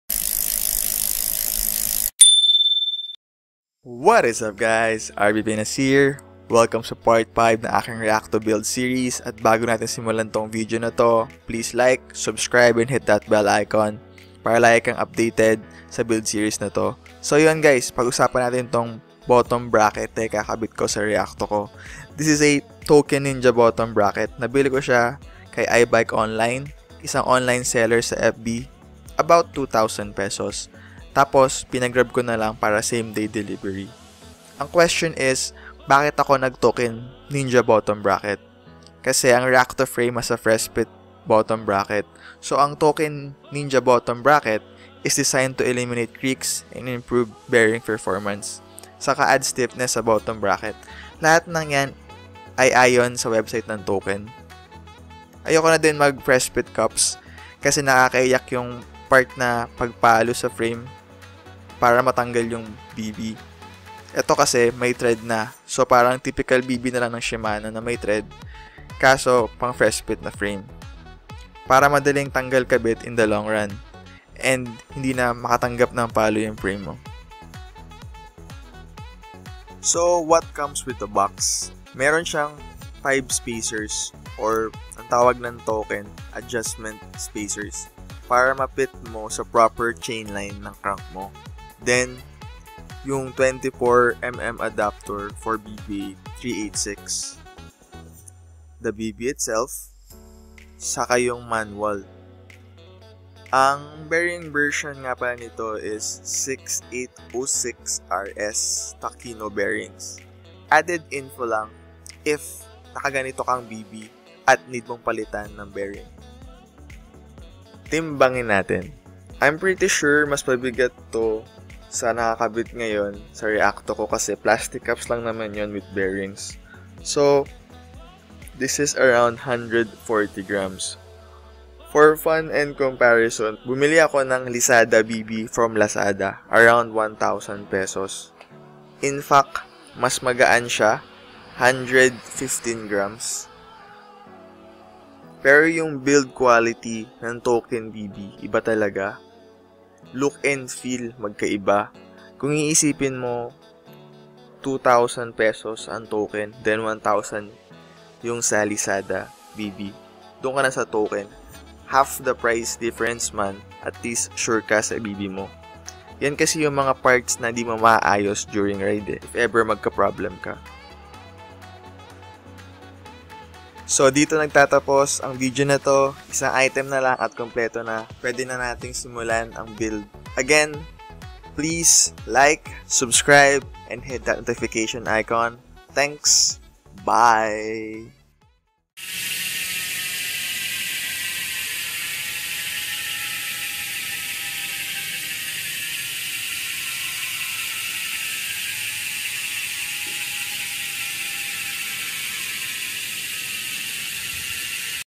Token Ninja Bottom Bracket | Reacto Build Series Part 5
What's up Guys another video na naman for this week! Welcome sa part 5 ng Reacto build series Pagusapan natin tong Token ninja BB na nabili ko kay ibike online. Thanks!